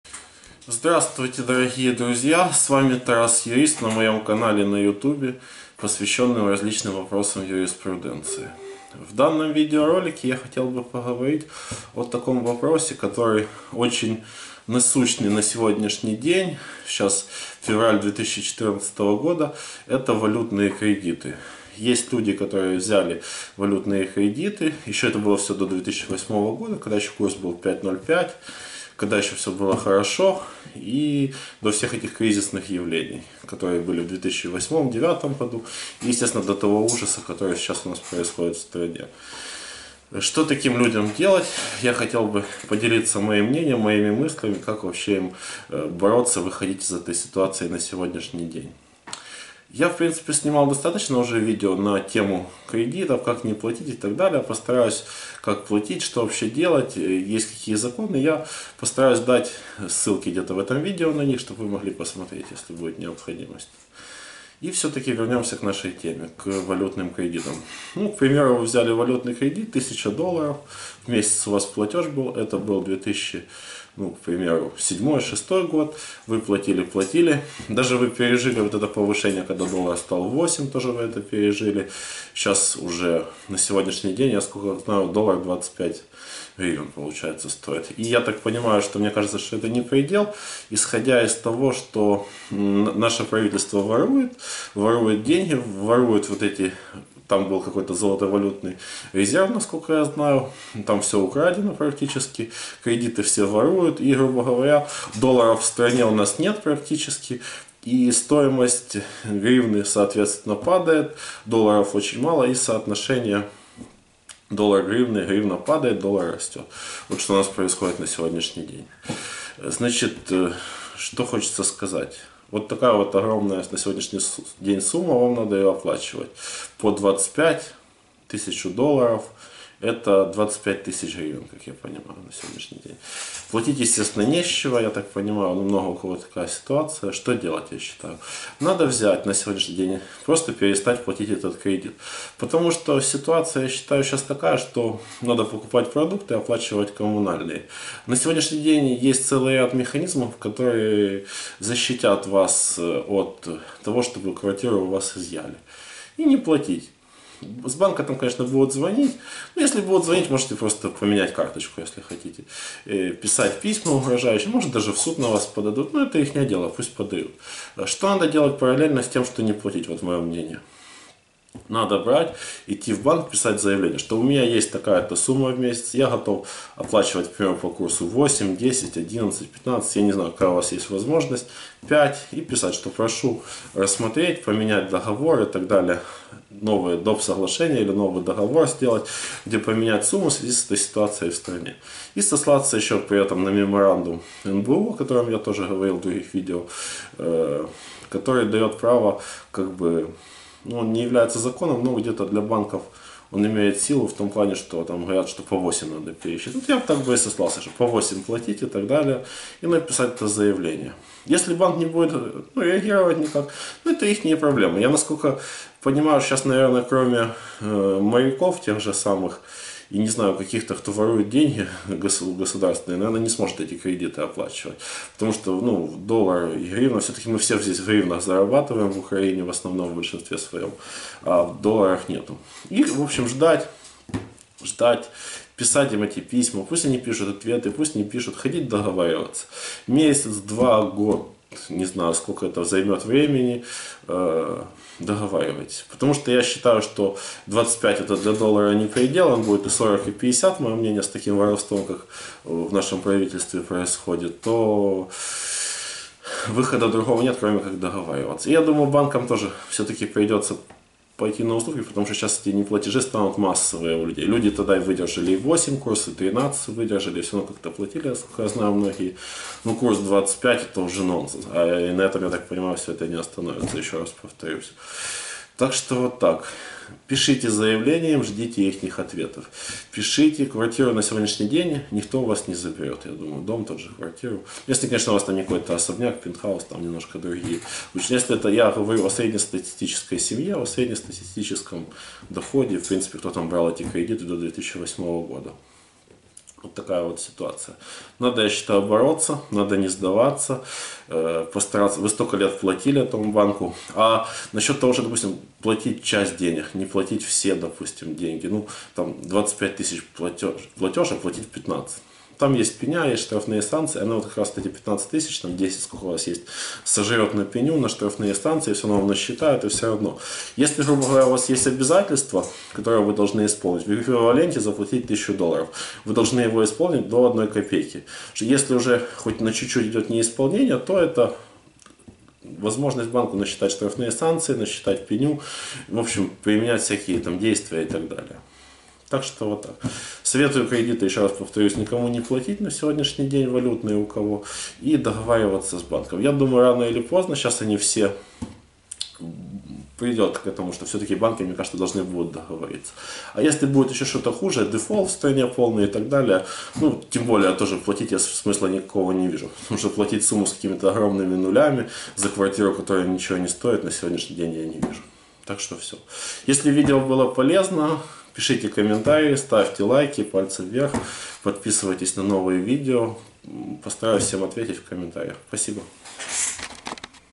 Валютный кредит 2015 Что делать?
На дворе февраль 2015 года, курс доллара вырос до неимоверных приделов и составляет 25 грн. за один доллар США. Многие доверчивые и не очень люди поддались на уговоры банковских сотрудников и получили ипотечные кредиты в долларах США, по курсу 5,05.
Валютный кредит в Украине в 2015 году стал настоящей бедой украинского народа. Я думаю, выход из этой ситуации на сегодня есть и он прост, надо перестать платить валютный кредит 2015 году? Считаю, если это сделают все граждане, то банкам не останется не чего другого кроме как договариваться с заемщиками. Из моего опыты могу заметить, что те кто платят валютный кредит в 2015 году, банк не захочет идти с ними на уступки и переговоры т. к., пока человек платит, то банку это подходит. Вот если человек перестает платить, тогда банк начнет задумываться о том, может стоит договариваться с заемщиком. Банк, это ведь не агентство недвижимости, ему нужны деньги, а не квартиры и дома граждан. Те более в современной ситуации, благодаря закону о моратории квартиру либо дом изъять без Вашего согласия практически не возможно.

Хотите получить ответ на вопрос валютный кредит 2015, что делать, смотрите это видео.